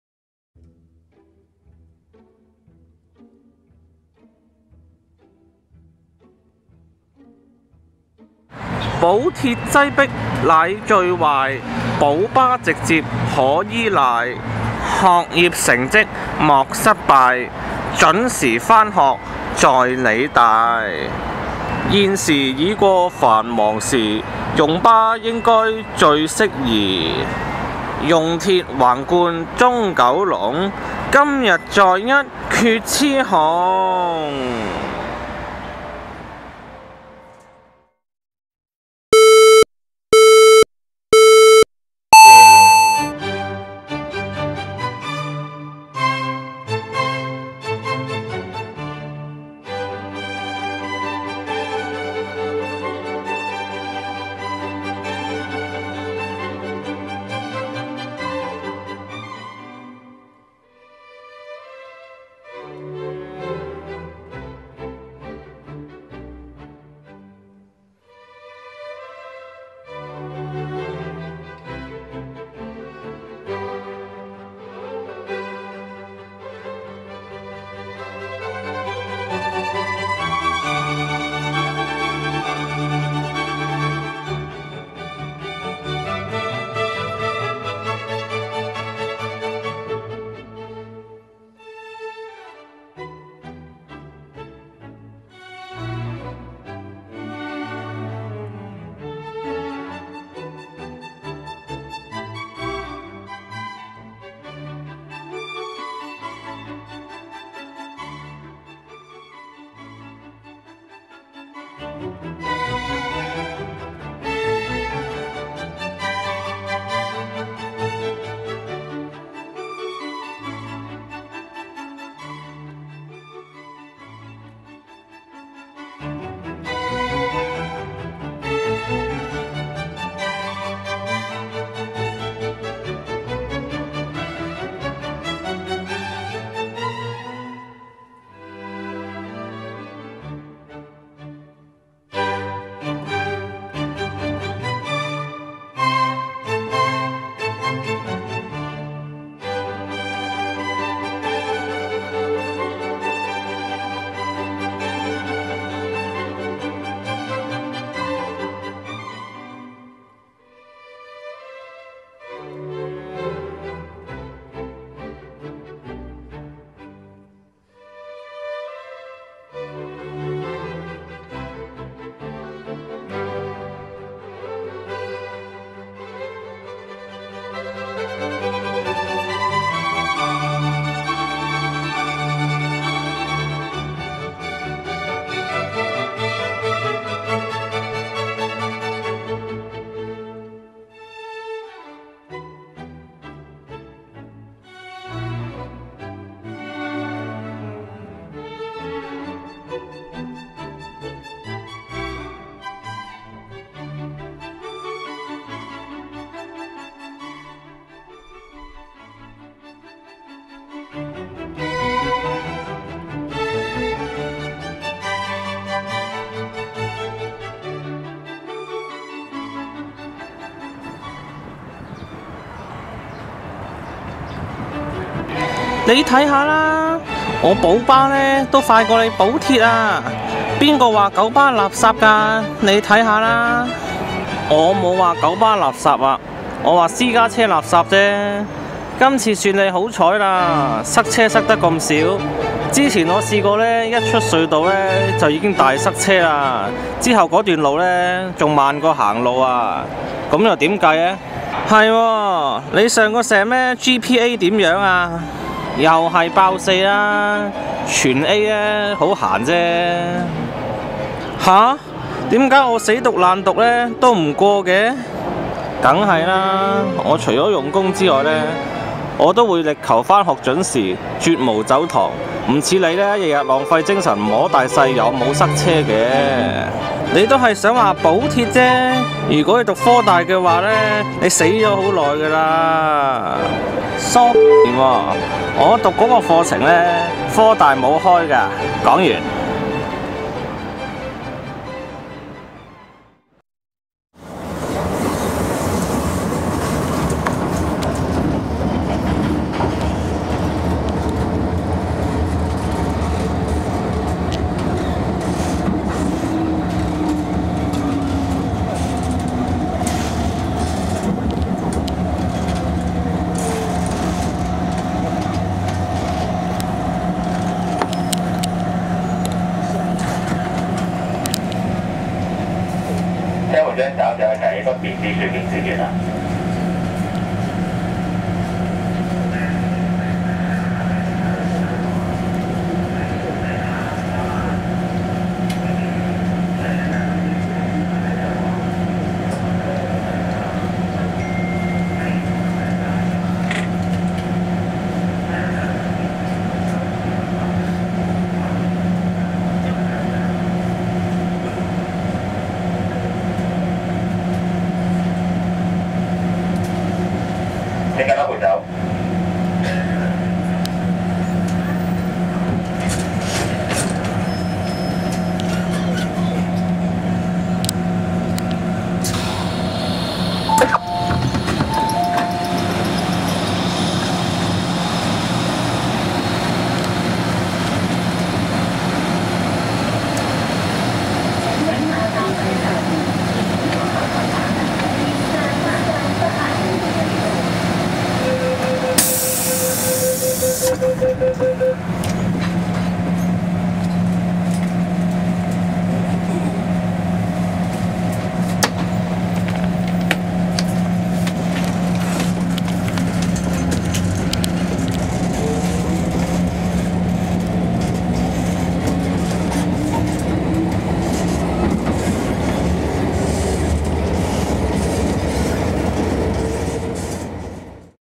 (Backup)【返學雌雄】藍田（匯景花園） → 理工大學 215X vs MTR
巴鐵一決雌雄
影片原製作者：藍田 武

起點：藍田匯景花園平台（近12/13座）
終點：理工大學平台（近E Core / Cafe）
使用巴士
狗巴路線215X
藍田站→理工大學
使用鐵路
MTR 觀塘線+東鐵
九龍塘轉乘

狗巴用時：31Min38Sec
講鐵用時：35Min30Sec

兩邊片段皆為12.5倍速度播送
即每4.8秒相等於實際上的一分鐘
彩蛋：地鐵車程期間在九龍灣站停車失準的情況

保鐵擠迫乃最壞
保巴直接可依賴
學業成績莫失敗
準時返學在理大
現時已過繁忙時
用巴應該最適宜
用鐵橫貫中九龍
今日再一決雌雄！

你看吧！
You see!
我「保巴」也比你「保鐵」快！
Taking the bus is faster than the train.
誰說「狗巴」是垃圾的？
Who claims that the KMB is crap?
你看吧！
You see!
我沒有說「狗巴垃圾」
I didn't mentioned that KMB is crap
垃圾的應該是「屍家車」
Instead, the private cars are crap.
這次是你好彩
You are so lucky at this time
塞車塞得這麼少
Having such little traffic jam.
上次我一離開隧道就已經大塞車
Last time, I encountered traffic jam just after leaving the tunnel
之後那段路比步行還要慢
During the jam, walking was even faster!
又何解？
How to explain that?
是的，你上學期的GPA（學業成績）如何？
Btw, what is your GPA last semester?
又是「爆四」，全部攞A很閒！
Easy job getting A grades in all subjects!
吓！為何我這麼用功也是不行的？
Why is my results so poor even I worked very hard?
當然！
Of course!
我除了用功之外
Besides working hard
我力求每天上學準時
I attend classes very punctually
完全沒有走堂
without skipping classes.
不像你這樣每天也浪費精神
Unlike you wasting your mind
「摸大細」有沒有塞車
thinking about traffic jams.
你也是想說「保鐵」吧！
Are you saying about the "godness" of railways?
如果你是讀科大（無鐵）的話
If you are studying in the HKUST(without railway)
你已經死掉了！
You would be already dead!
So99y
我讀你這個課程科大沒有
The degree that I'm studying is not offered by HKUST
講完！
That's the end!

此片段與因管理不善而導致二零一二至一三年間有多名巴膠對異見人士進行批鬥、起底行為；
有管理猿（姓譚）實行大細超、偏坦同黨人士、封殺異見人士；
以及頑強無比，至二零一六年仍未關門大吉，
期間導致大量青少年及兒童被灌輸巴膠意識，
香港社會鐵路意識遠遠落後於其它東洋國家，
為害不淺的畜生地獄式香港巴膠大典、
以及某「215X大帝」並沒有任何關係，敬請留意。

多謝! Thank You!